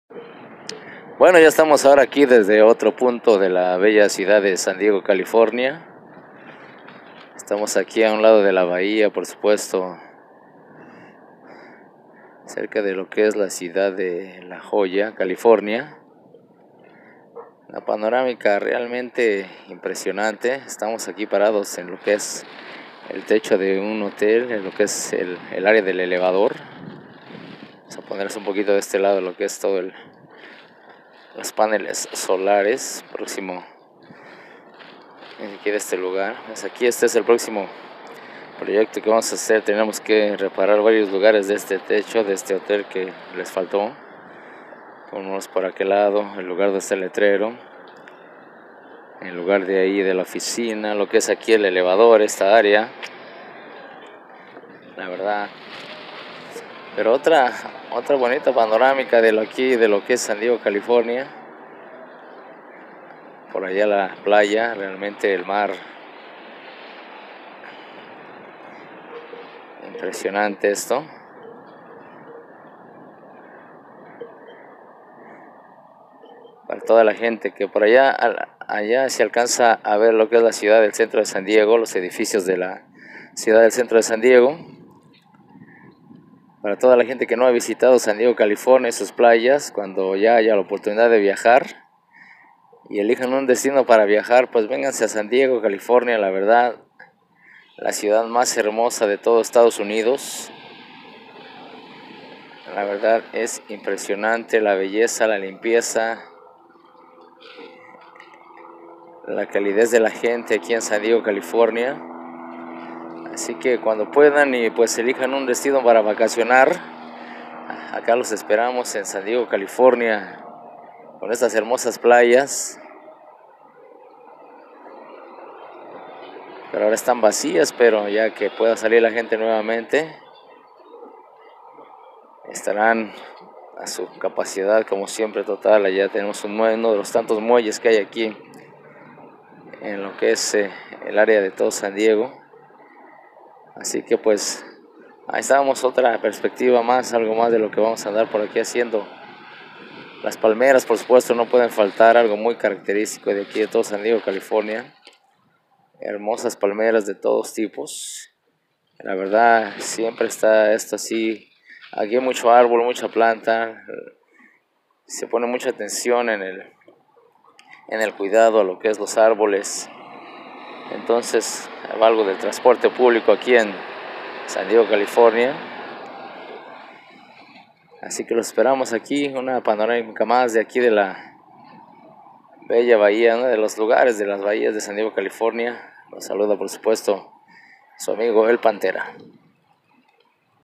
VISTA PANORAMICA DE LAS PLAYAS DE SAN DIEGO CA.
LAS PLAYAS DE LA JOLLA EN SAN DIEGO CA DESDE UN ELEVADOR.